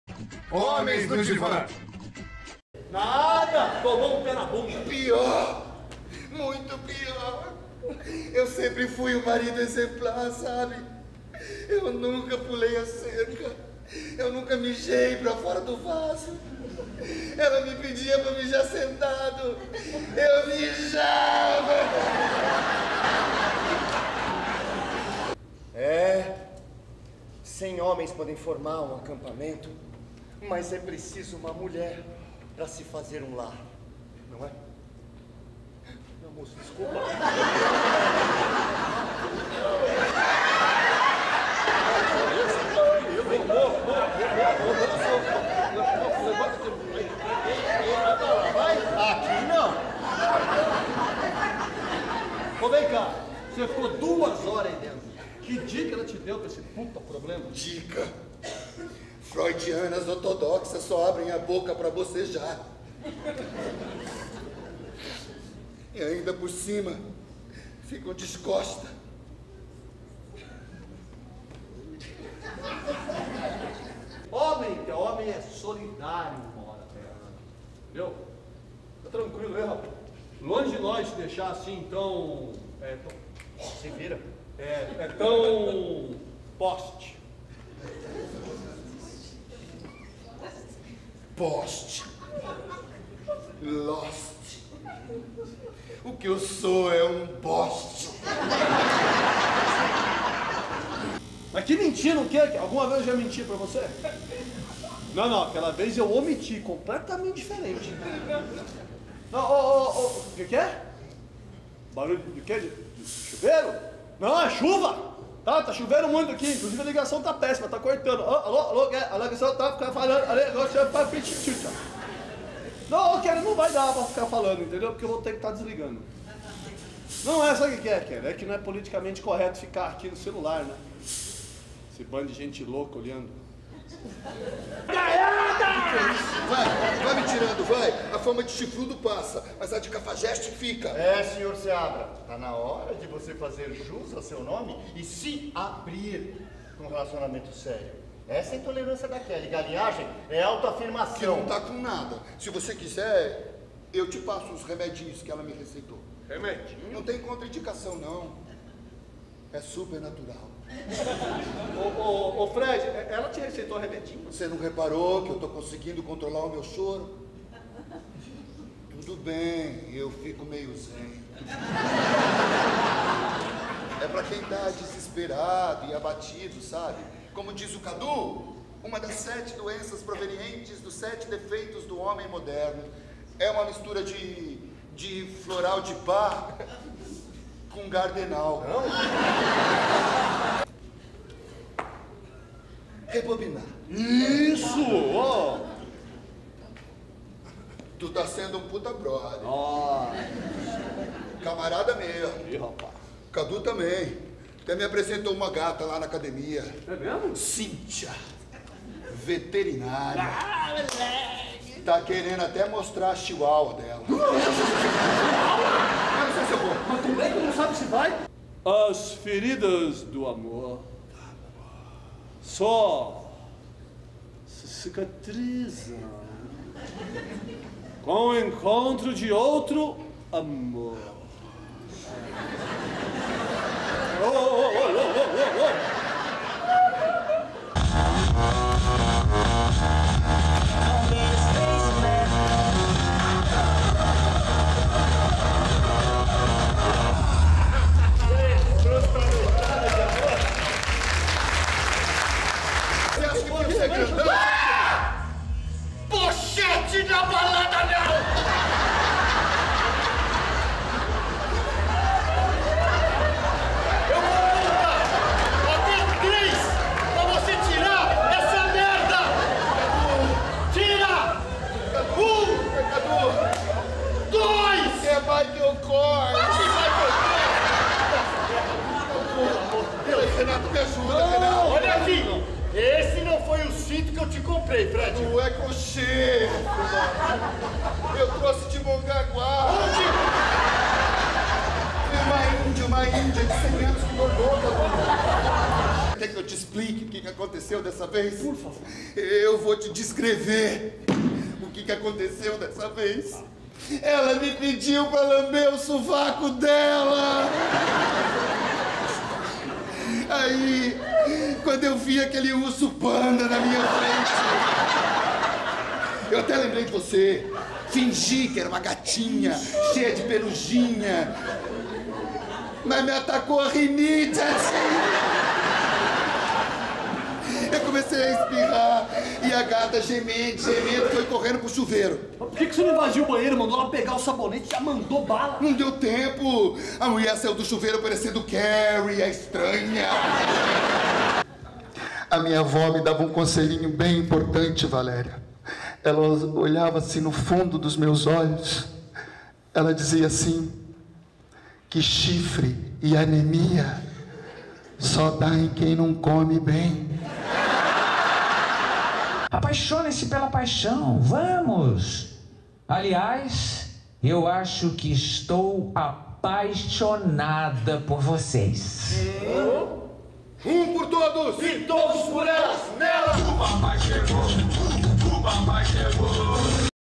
HOMENS NO DIVÃ - COMÉDIA IMPERDÍVEL!
HOMENS NO DIVÃ comédia com Guiherme Chelucci, Ken Kadow e Darson Ribeiro, texto Miriam Palma DIREÇÃO GERAL Darson Ribeiro DE 03/06 a 10/07/22 Sextas e Sábados às 20h e Domingos às 19h TEATRO DAS ARTES Shopping Da Gávea